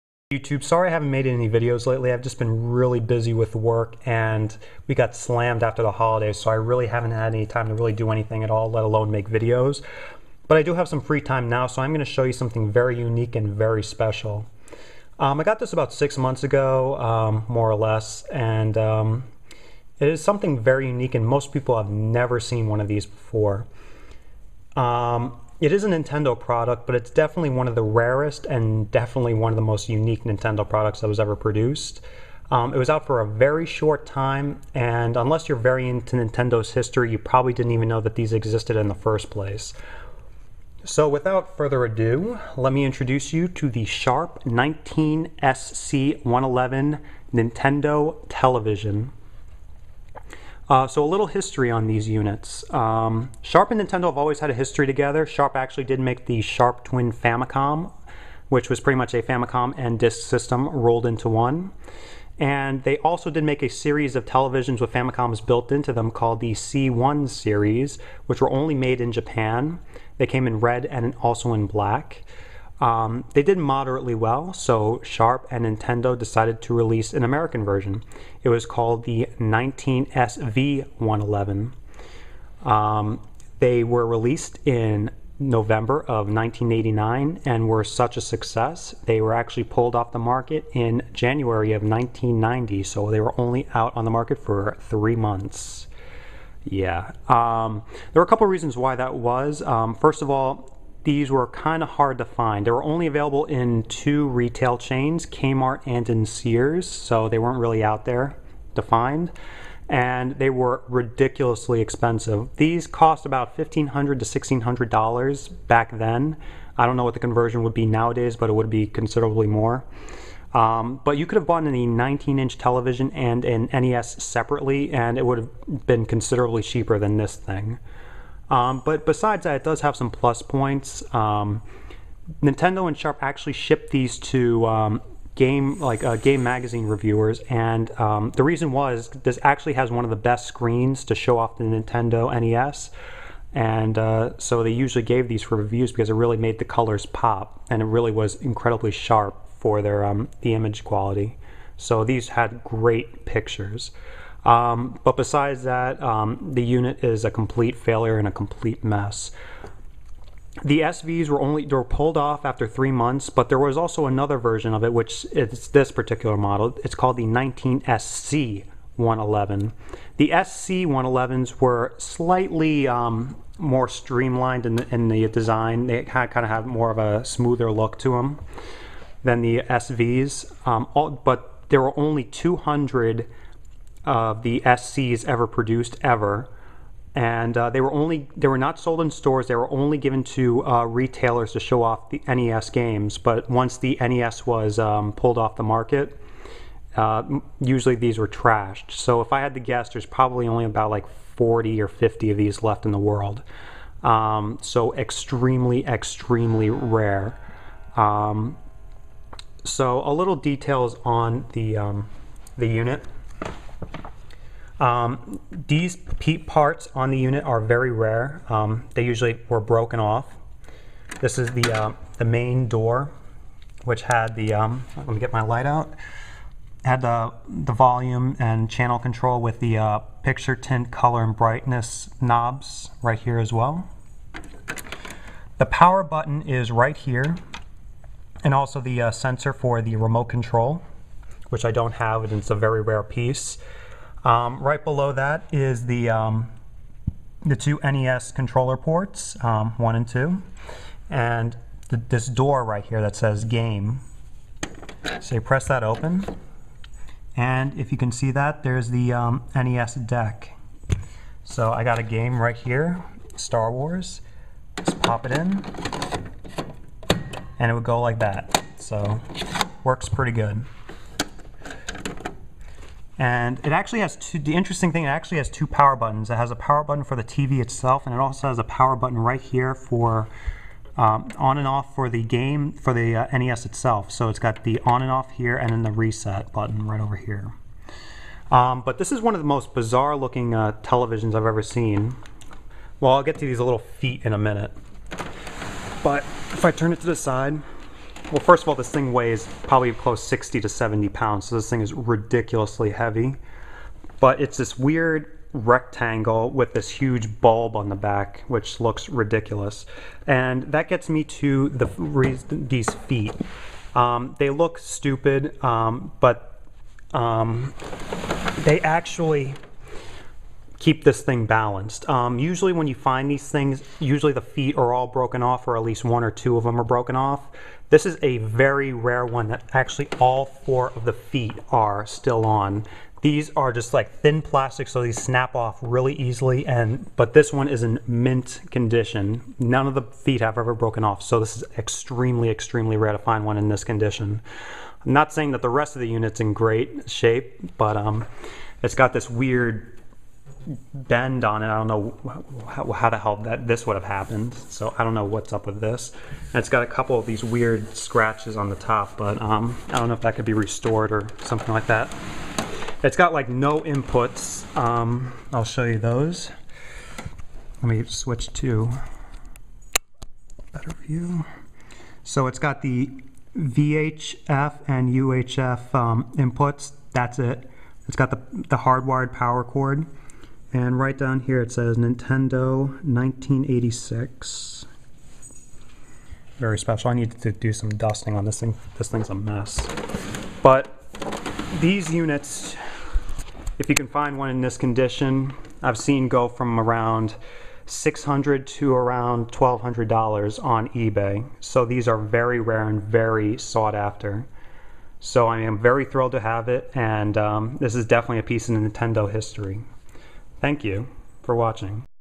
Ultra Rare Sharp Nintendo NES TV!!!
A very rare and unique piece of Nintendo's history. Sharp 19SC111 Nintendo Television